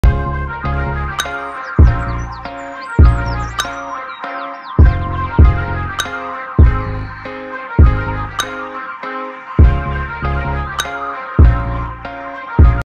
Coleman vs Bromell 💨 Who’s start is better?
Slow motion video of Christian Coleman vs Trayvon Bromell coming out of the blocks.